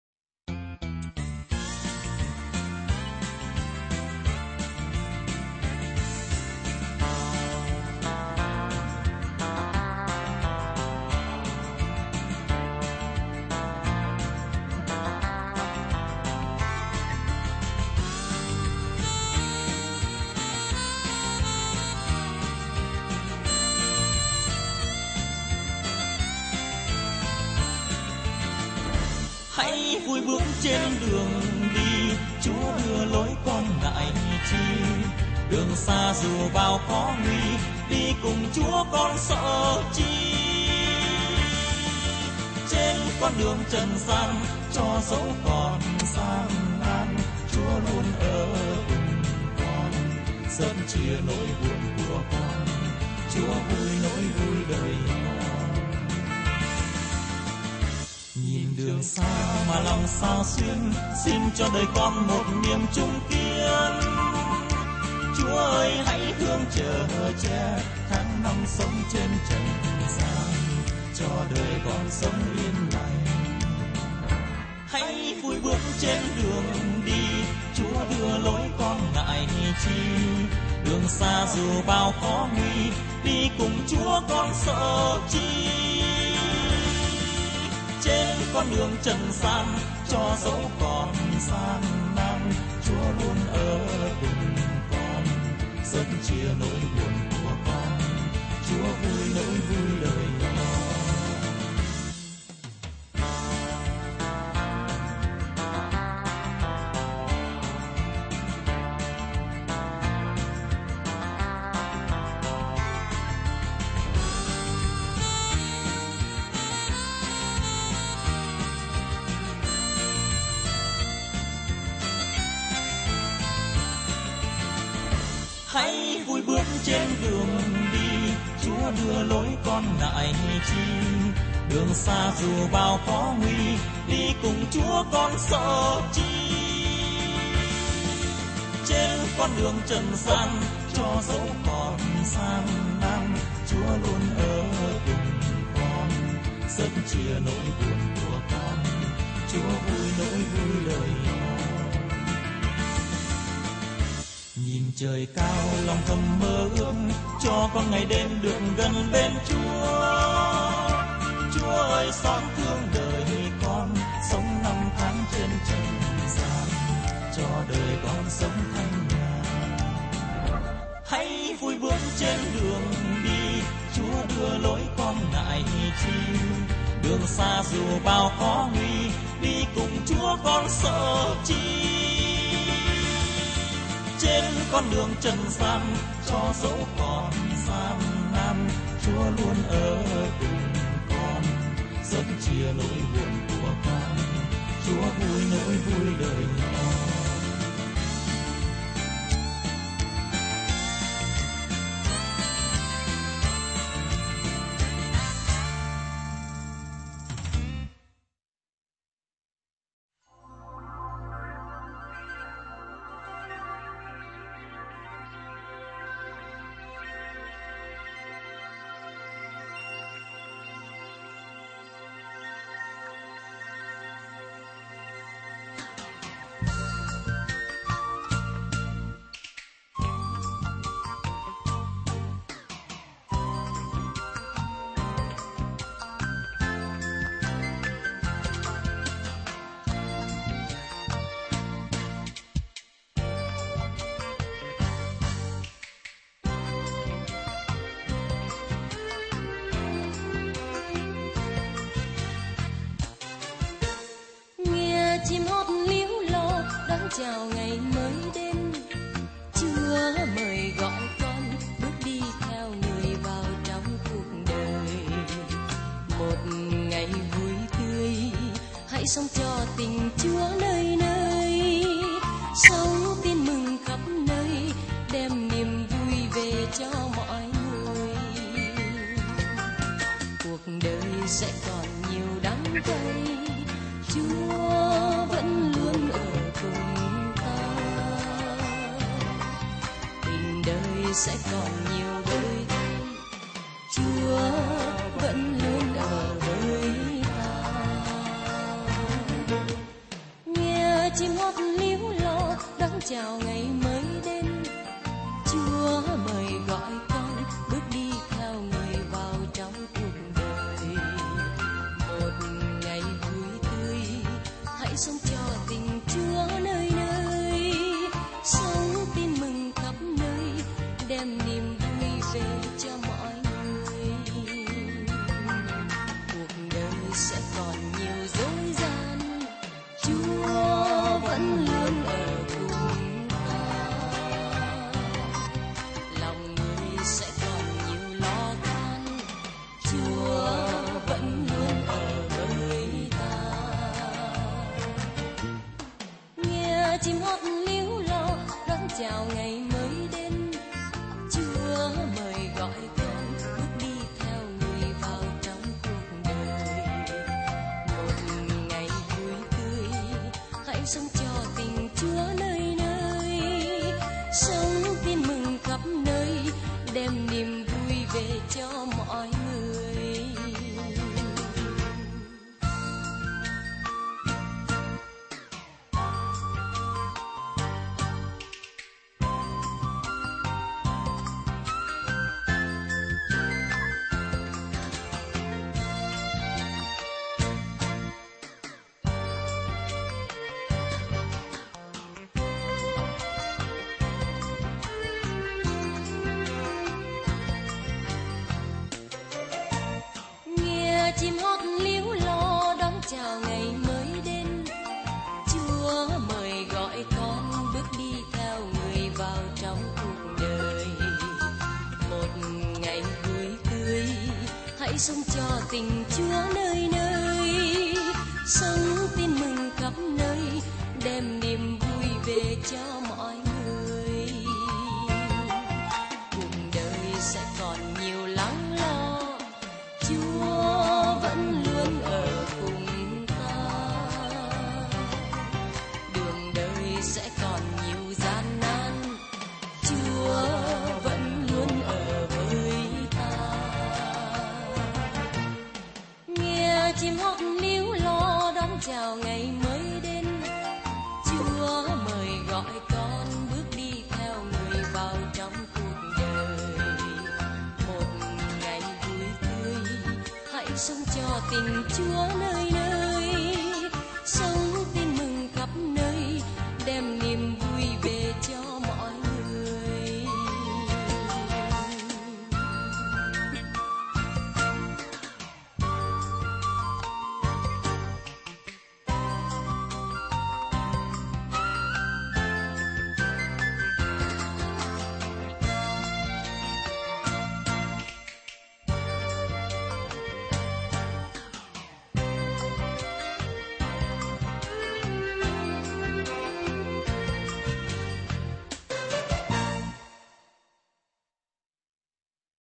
Duc TGM Phero Nguyen Van Tot - Su Than Toa Thanh ve tham que huong-22-8-2012-DVD 2 .wmv
Duc Tong Giam Muc Phero Nguyen Van Tot - Su Than Toa Thanh tai Costa Rica ve tham que huong ngay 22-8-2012.